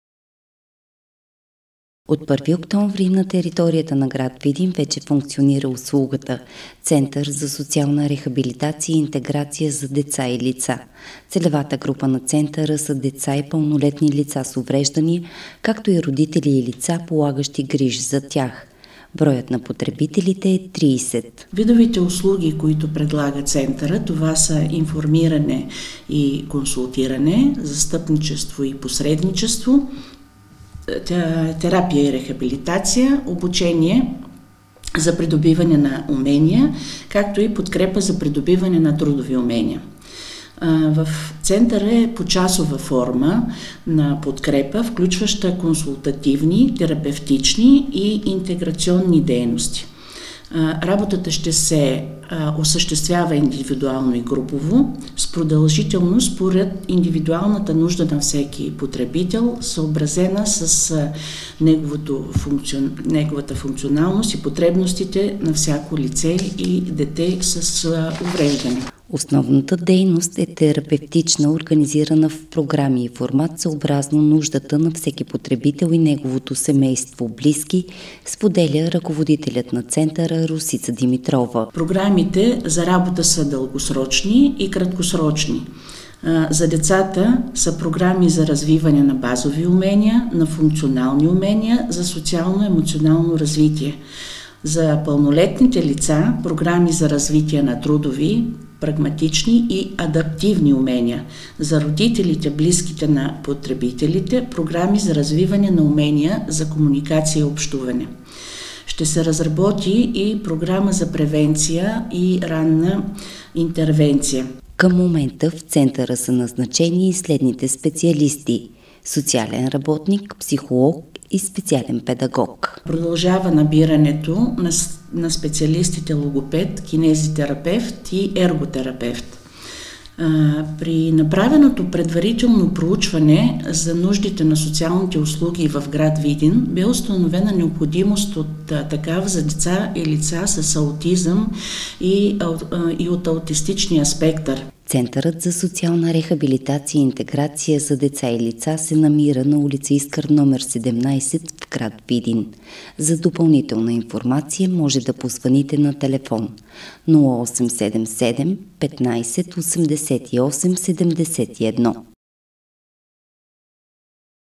Нова социална услуга в община Видин
От началото на този месец функционира услугата „Център за социална рехабилитация и интеграция за деца и лица”. Тя се управлява чрез Общинското предприятие „Социални дейности”.